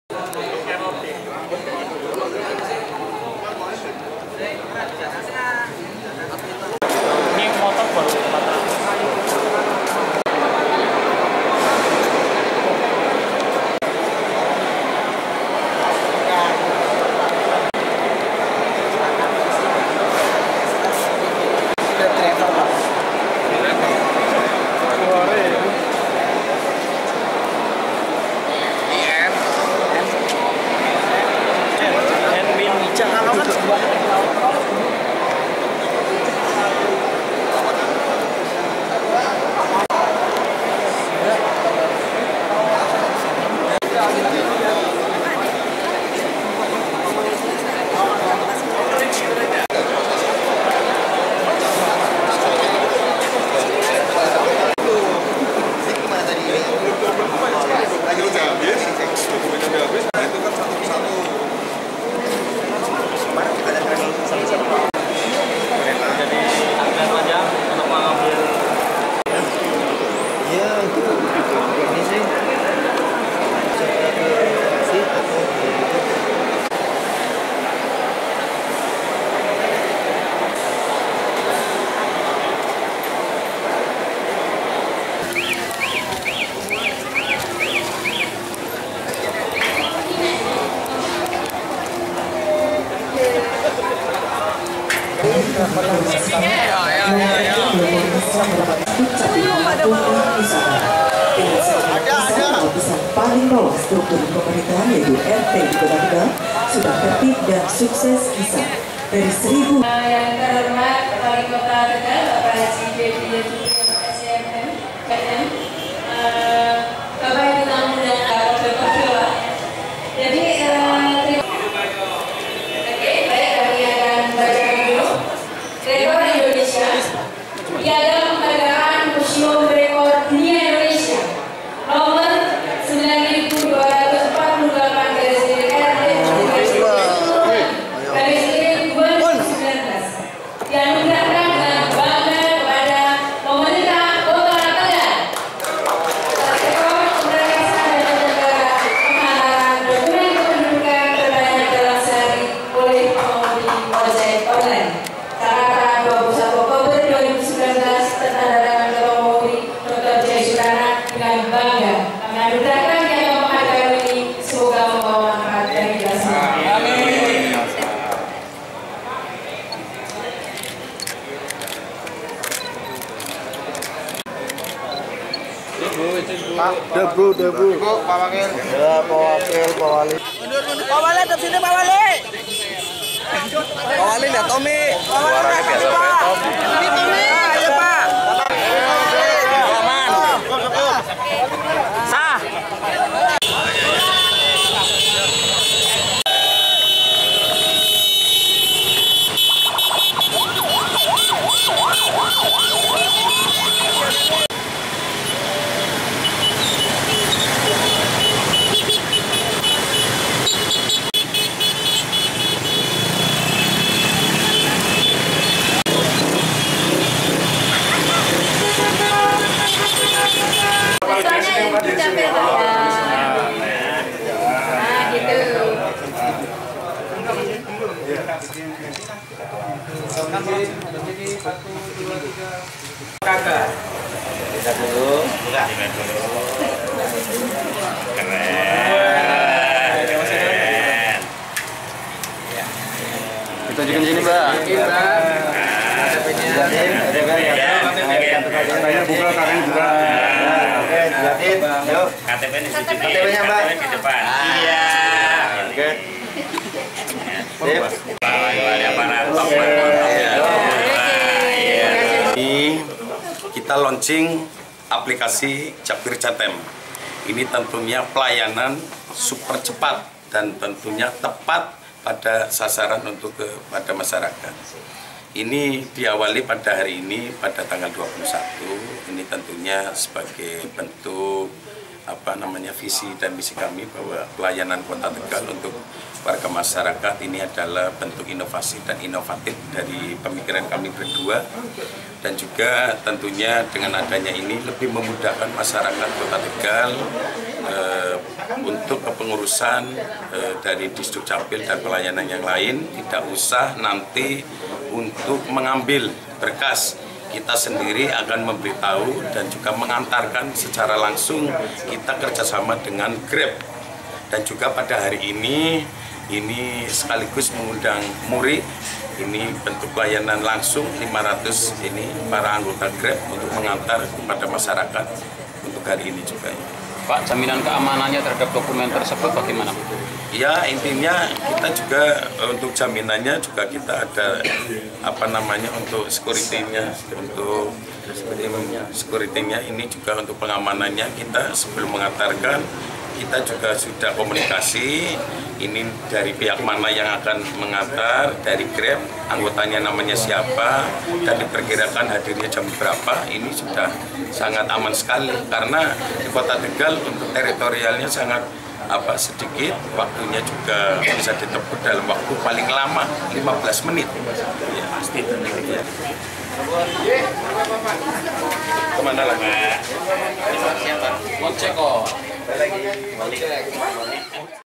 Rekor Muri Diraih Pemkot Tegal, Karena Lakukan Ini
Pemecahan Rekor Muri kategori layanan dan pengiriman dokumen kependudukan terbanyak berhasil dilakukan Dinas Kependudukan dan Pencatatan Sipil Kota Tegal, bekerja sama dengan ojek daring, Senin (21/10/2019) siang.

Disdukcapil melibatkan sedikitnya 485 mitra ojek daring untuk mengantarkan dokumen kependudukan kepada pemohon. Layanan ini bisa dilakukan, setelah pemohon mengakses program inovasi layanan aplikasi administrasi kependudukan bernama Jakwir Cetem (Aja Kosih Wira-Wiri, Cepat dan Tepat Melayani).